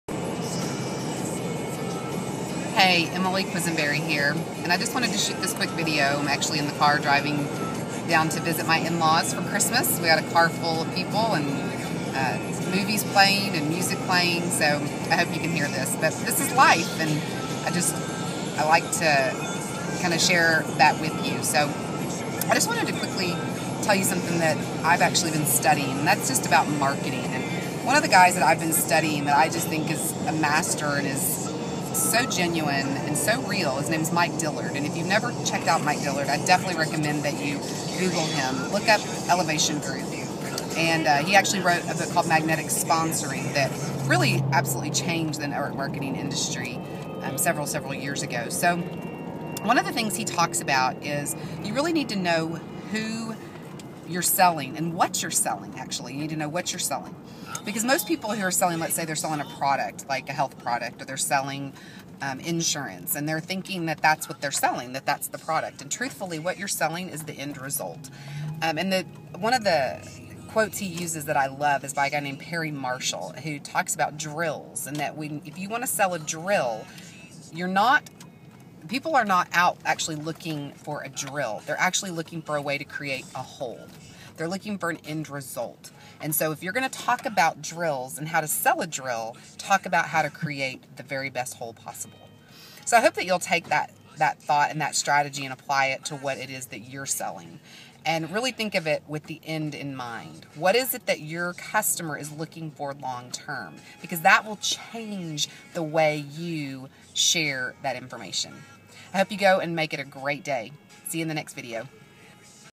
Successful MLM Marketing Strategies | How to Market and Sel
Successful MLM Marketing Strategies | How to Market and Sell Effectively

Often people who are selling a product or service only talk about the features and benefits of what they are selling.  They never stop to think what it is the customer is looking for long term.  If you are selling a health product, your customer isn't really purchasing the type of product or the amount of times per day you take it or the ingredients.  They are purchasing a quality of life or a long term result.  When you make this shift, you will be amazed at the results you get from your MLM marketing.